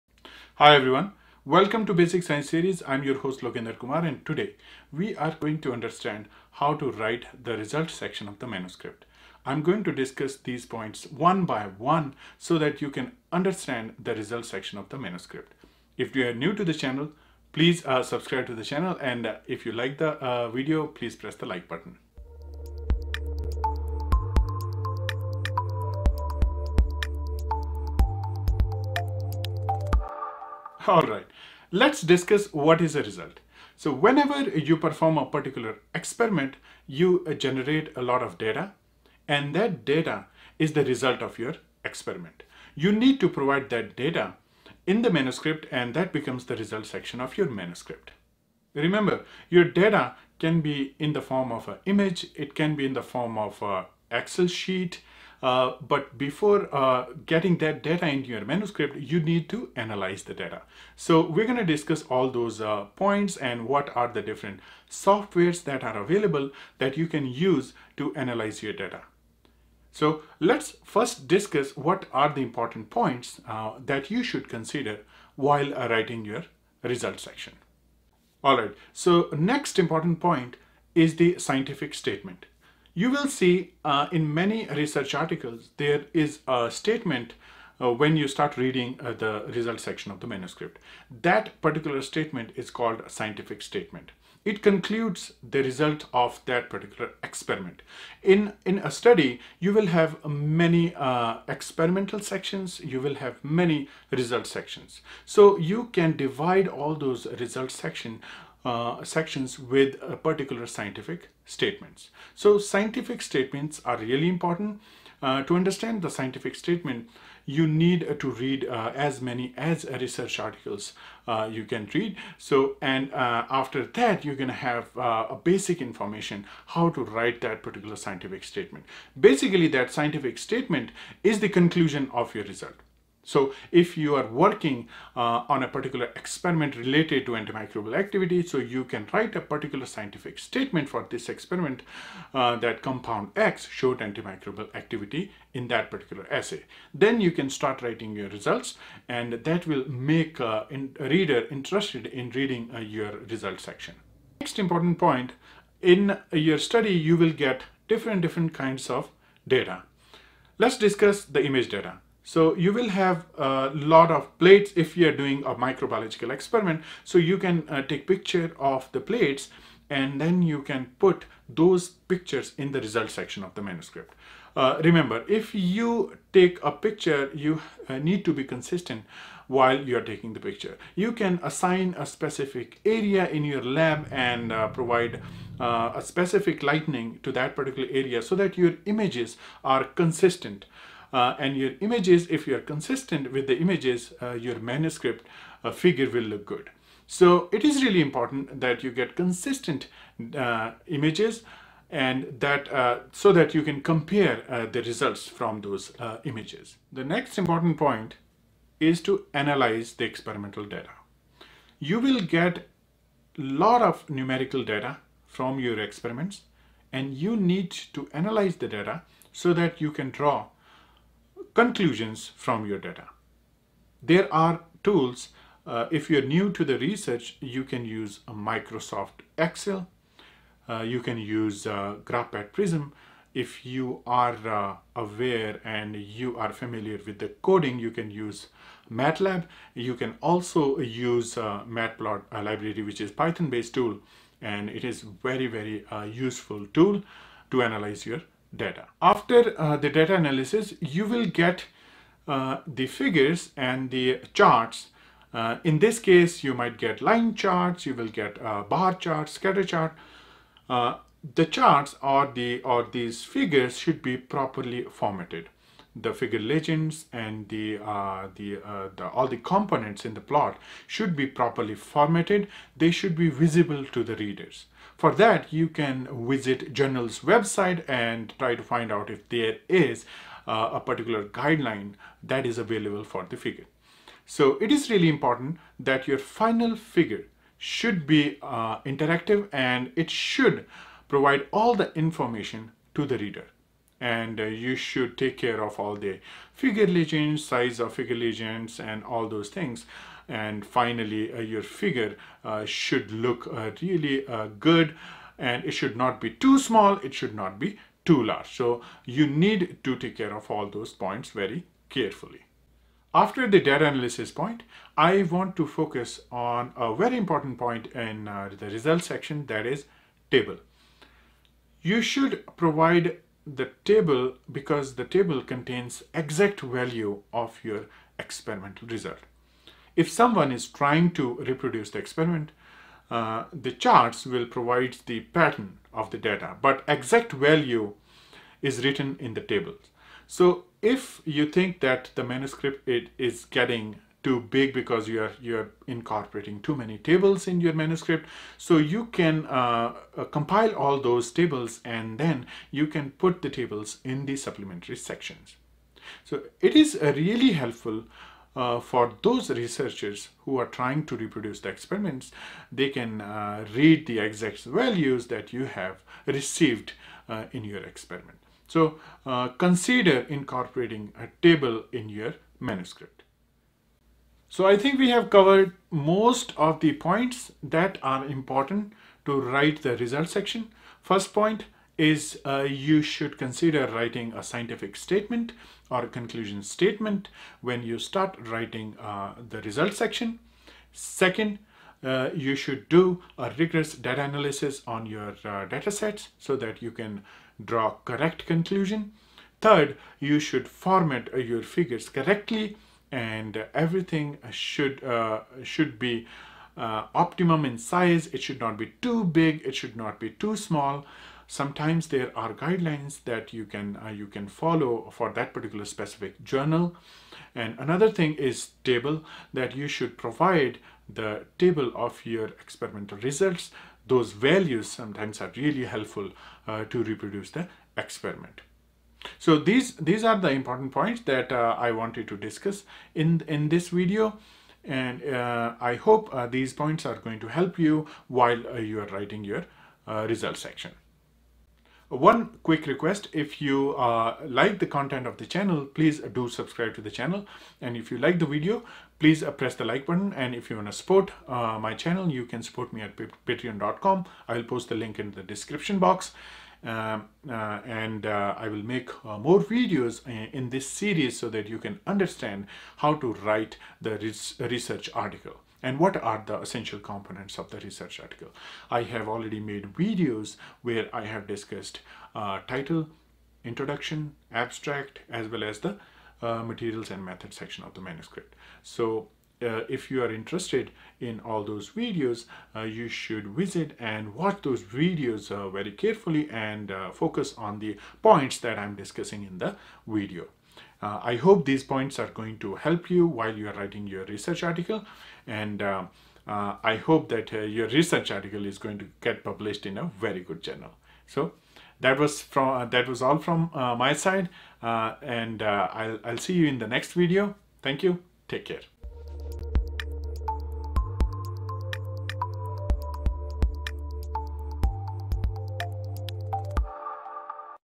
How to write a Result Section in Research Paper| Manuscript | Part-7 | Basic Science Series
I have discussed how to right the result section of the manuscript in the area of biosciences. I have explained what is data? what are results and how to analyze data to interpret the results? I have explained what is the software you can use to analyze data and present the data. I have explained the guidelines to collect image data. In addition, I have discussed image data collection in the laboratory and also draw proper conclusions from the data. I have explained the importance of the tables in the file and how you can use the supplementary section to provide all the data from your experiment. I wish everyone a great writing session and I hope this video will help you to write the good result section of the manuscript.

Experimental data in science and engineering is data produced by a measurement, test method, experimental design, or quasi-experimental design. In clinical research, any data produced are the result of a clinical trial. Experimental data may be qualitative or quantitative, each being appropriate for different investigations.

Generally speaking, qualitative data are considered more descriptive and can be subjective in comparison to having a continuous measurement scale that produces numbers. Whereas quantitative data are gathered in a manner that is normally experimentally repeatable, qualitative information is usually more closely related to phenomenal meaning and is, therefore, subject to interpretation by individual observers.